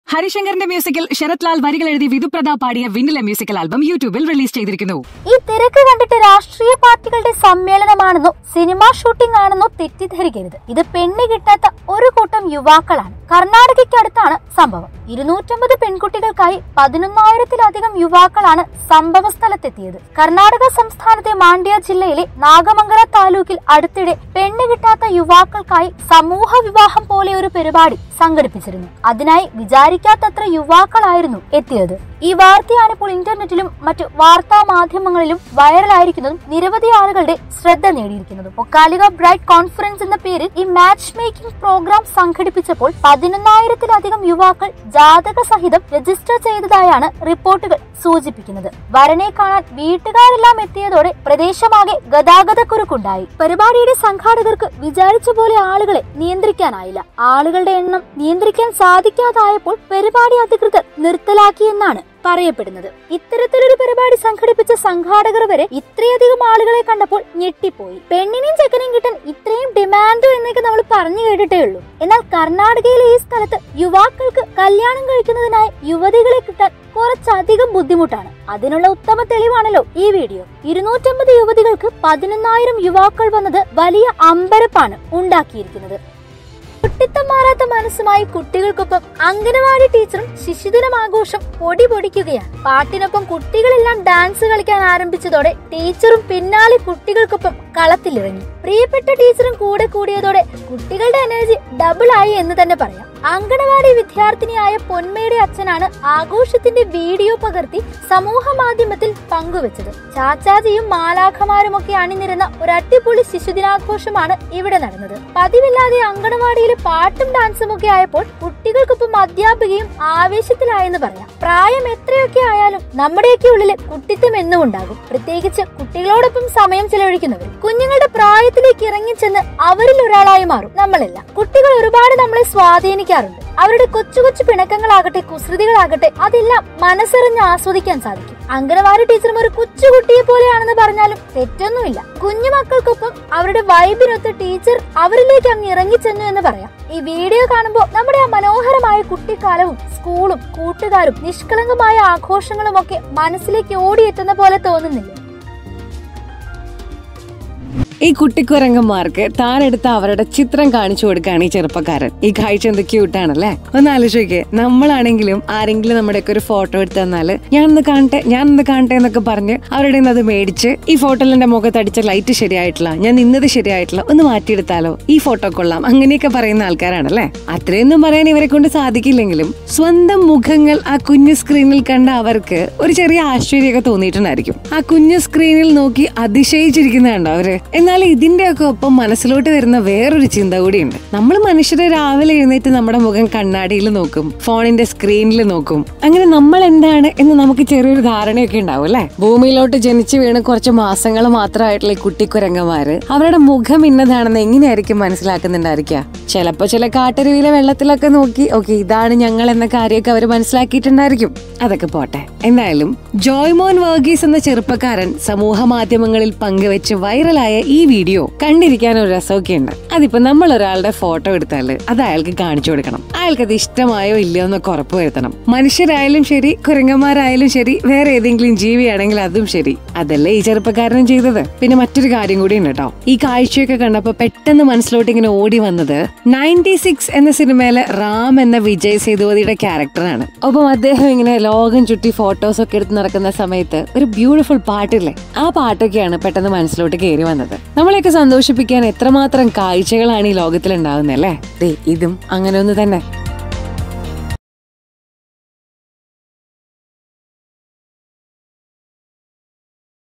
250 പെൺകുട്ടികളെ വിവാഹം കഴിക്കാനായി MATCH MAKING EVENTൽ പങ്കെടുക്കാനെത്തിയത് 11000 ചെറുപ്പക്കാർ
നമ്മുടെ നാട്ടിൽ പെണ്ണ് കിട്ടാത്ത ഇത്രയധികം ചെറുപ്പക്കാരുണ്ട് 😱 250 പെൺകുട്ടികളെ വിവാഹം കഴിക്കാനായി MATCH MAKING EVENTൽ പങ്കെടുക്കാനെത്തിയത് പതിനായിരത്തിലധികം ചെറുപ്പക്കാർ